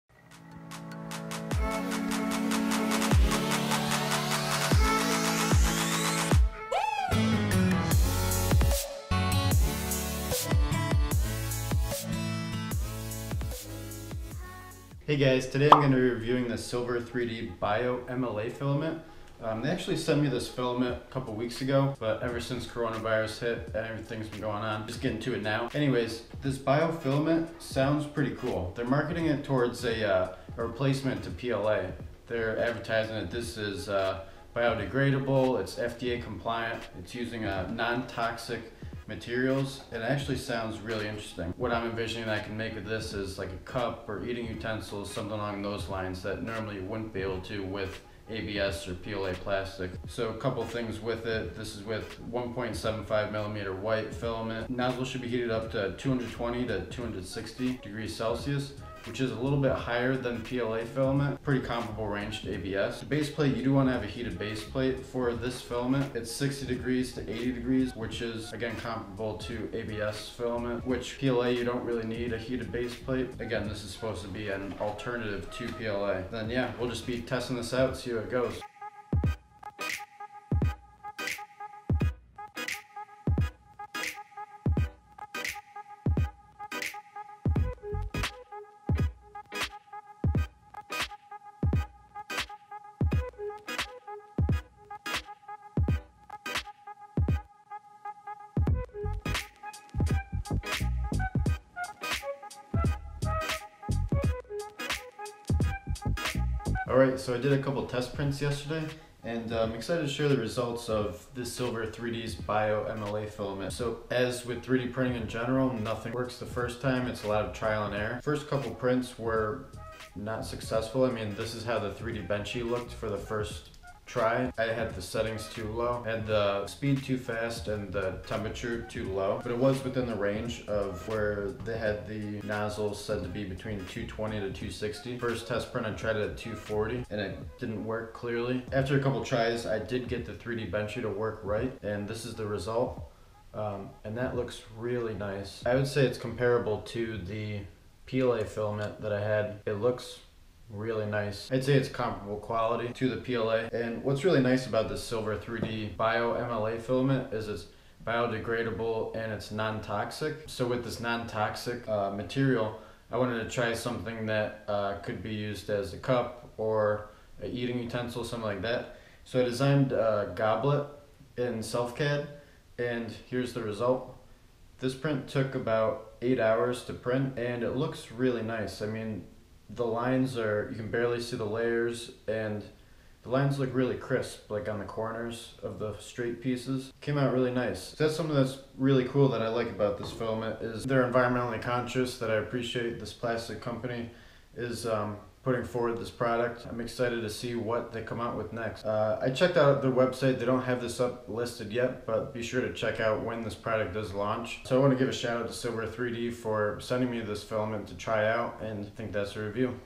Silver 3D Bio-MLA Filament Review!
Quick review of a new type of filament that Silver3D sent me to try out!
Bio-MLA is biodegradable and non-toxic so that sounded really appealing.
Overall the results were pretty good!
The only negatives I saw were that there clearly is a learning curve when trying out new filaments as I’m so used to my settings I already have dialed in for my usual filaments.
For people new to 3d printing this filament may be tricky to print as you have to manually adjust your slicer settings.
I ran the nozzle at 260 and print speed I really had to slow down to 40mm/s which meant a longer print time than normal.
Even though it’s a longer print time I think the positives of it being environmentally friendly is a great alternative! Also a huge plus that there wasn’t any foul odor like when I’m printing with ABS.
The goblet I showed in the video was designed using SelfCAD!
According to the specs the Bio-MLA works well with acrylic paints. I’ll have to do a follow up post at some point once I paint it!👑🎨
You can find the goblet I designed free to download over at MyMiniFactory!